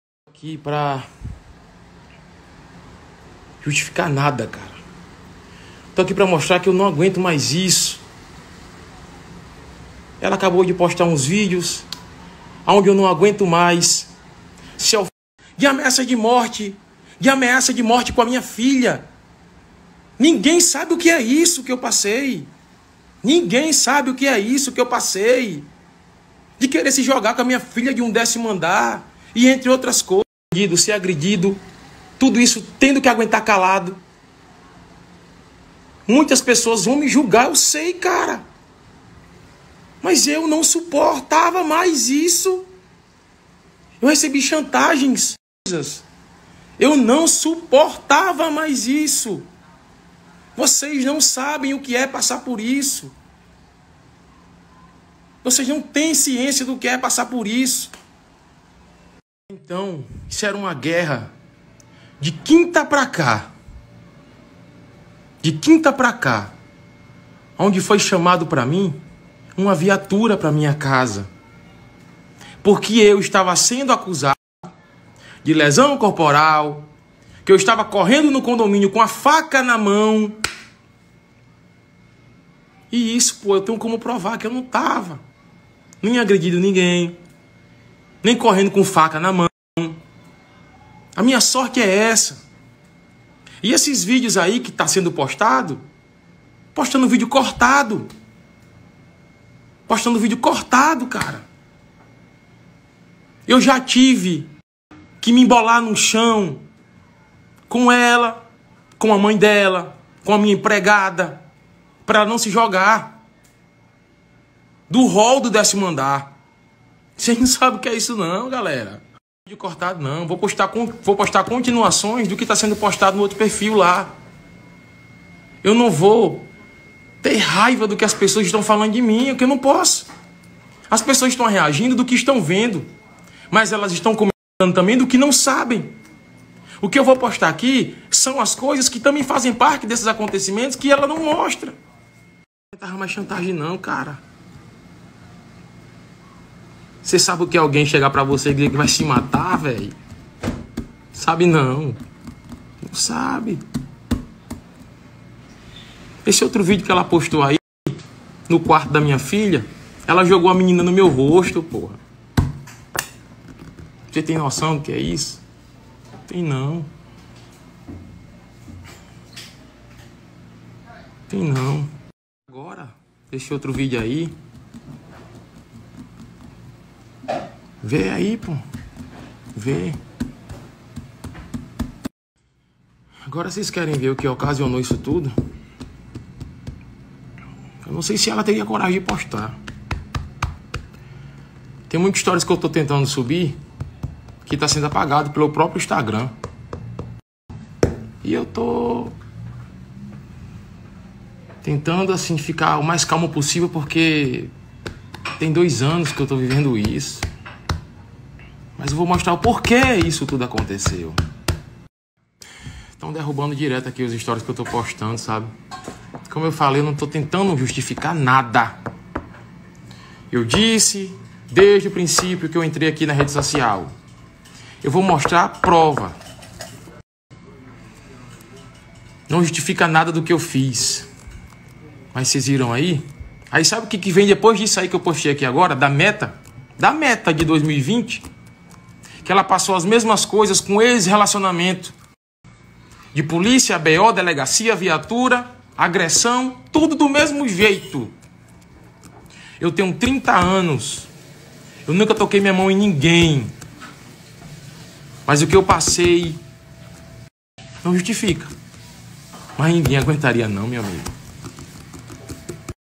DJ IVIS DIZ QUE VINHA SOFRENDO ATÉ AMEAÇA DE MORTE..
O DJ fez um vídeo se pronunciando sofre o fato ocorrido na noite deste domingo..